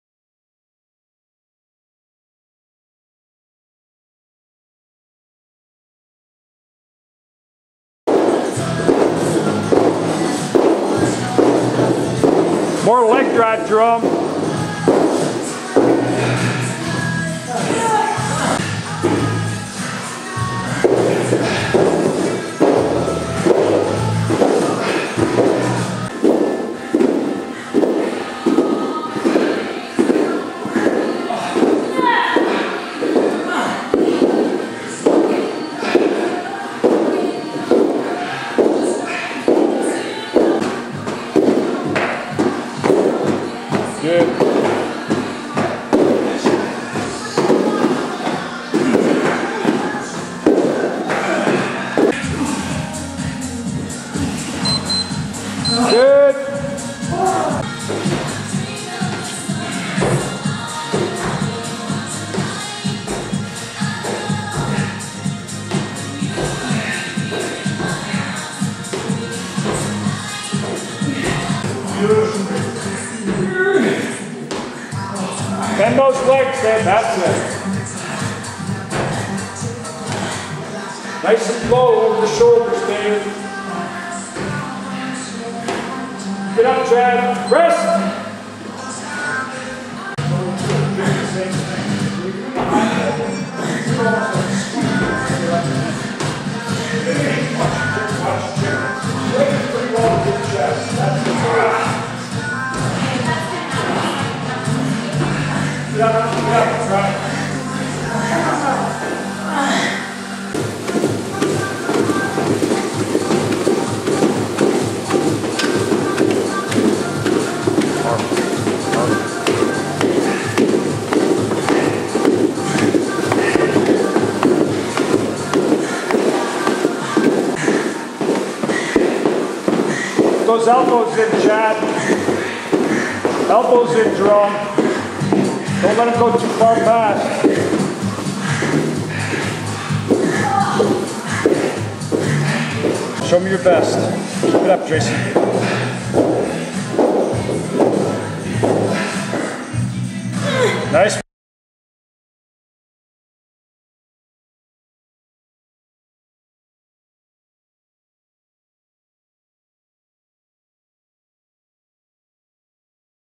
BMSfit Kettlebell, Club, and Rope Interval Station Circuit Wednesday Oct 18, 9:30 a.m.
We used a variety of equipment today. The stations we included were, Chin ups, Box jumps, Club Swipes, Kettlebell High pulls, Uneven surface push ups and Battling ropes. Was a great session all day here you see the 9:30 a.m. class mix it up.